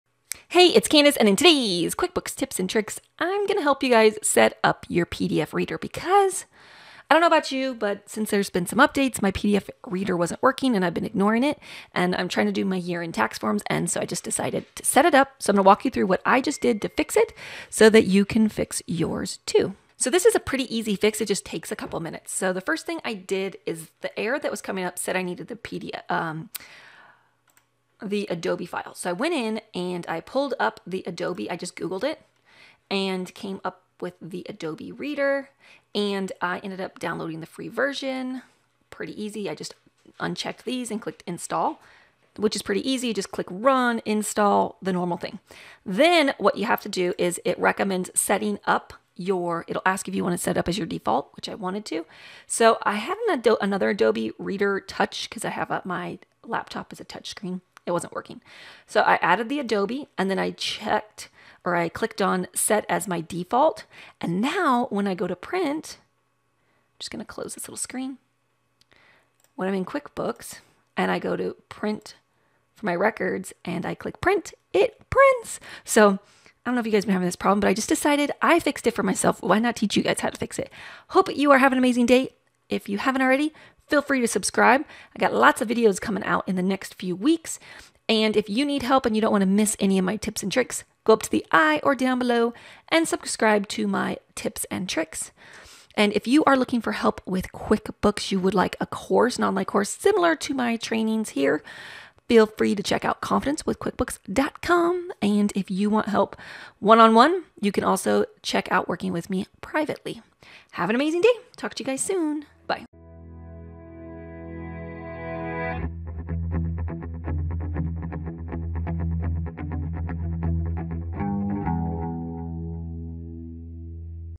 How to fix PDF error in QuickBooks
Getting an error when trying to print a PDF/Report out of QuickBooks? 😳

Let me show you how to fix that in QuickBooks…

In this video, you will learn How to fix PDF error in QuickBooks.

➡️ Steps to Fixing PDF Error:

Google “Adobe Acrobat Reader”
Download the “Free version”
Install it
“Set your default programs”
Choose “Adobe Acrobat Reader DC”
Click “Set this program as default”
Print your PDF in QuickBooks

Timestamps:

0:00 - Intro
0:20 - Update Adobe reader
1:20 - Final Thoughts